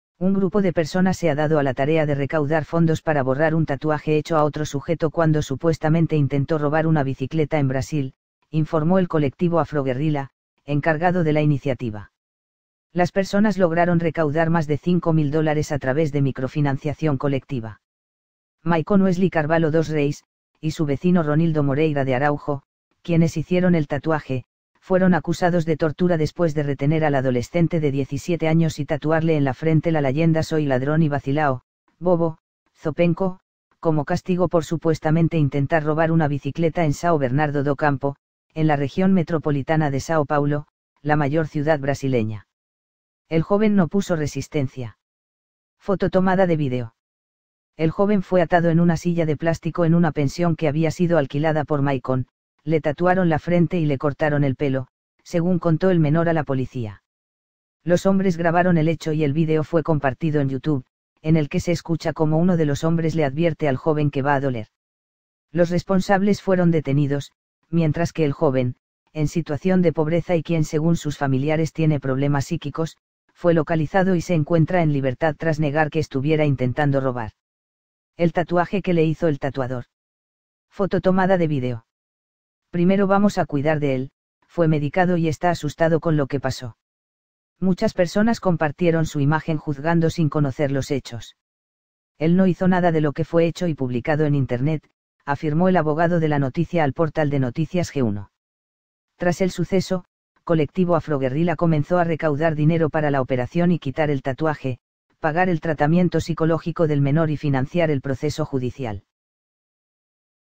Recaudan dinero para borrar tatuaje de presunto ladrón!
- Un grupo de personas se ha dado a la tarea de recaudar fondos para borrar un tatuaje hecho a otro sujeto cuando supuestamente intentó robar una bicicleta en Brasil, informó el Colectivo Afroguerrilha, encargado de la iniciativa.

Las personas lograron recaudar más de cinco mil dólares a través de microfinanciación colectiva.
Maycon Wesley Carvalho dos Reis, y su vecino Ronildo Moreira de Araujo, quienes hicieron el tatuaje, fueron acusados de “tortura” después de retener al adolescente de 17 años y tatuarle en la frente la layenda “soy ladrón y vacilão (bobo, zopenco)" como castigo por supuestamente intentar robar una bicicleta en Sao Bernardo do Campo, en la región metropolitana de Sao Paulo, la mayor ciudad brasileña.

El joven no puso resistencia. Foto tomada de video
El joven fue atado en una silla de plástico en una pensión que había sido alquilada por Maycon, le tatuaron la frente y le cortaron el pelo, según contó el menor a la Policía.
Los hombres grabaron el hecho y el video fue compartido en YouTube, en el que se escucha cómo uno de los hombres le advierte al joven que va a doler.
Los responsables fueron detenidos, mientras que el joven, en situación de pobreza y quien según sus familiares tiene problemas psíquicos, fue localizado y se encuentra en libertad tras negar que estuviera intentando robar.

El tatuaje que le hizo el tatuador. Foto tomada de video
"Primero vamos a cuidar de él, fue medicado y está asustado con lo que pasó. Muchas personas compartieron su imagen juzgando sin conocer los hechos. Él no hizo nada de lo que fue hecho y publicado en internet", afirmó el abogado de la noticia al portal de noticias G1.
Tras el suceso, Colectivo Afroguerrilha comenzó a recaudar dinero para la operación y quitar el tatuaje, pagar el tratamiento psicológico del menor y financiar el proceso judicial.